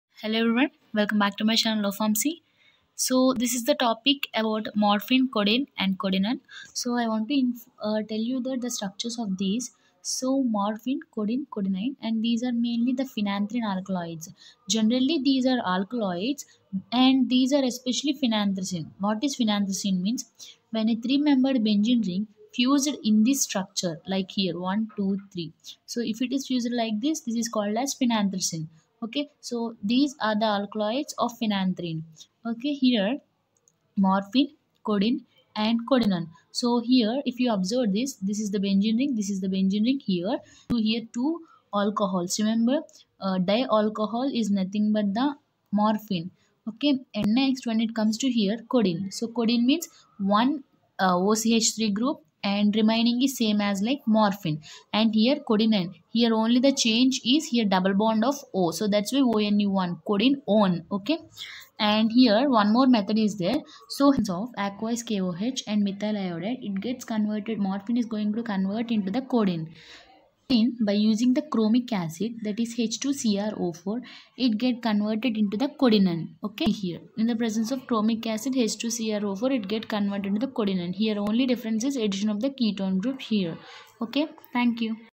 structure of morphine,codeine,codeinone,(conversion of morphine to codeine,and codeinone)in detail.
the complete details of morphine, codeine, codeinone in structure.
the chemical reactions of morphine to codeine by using reagents.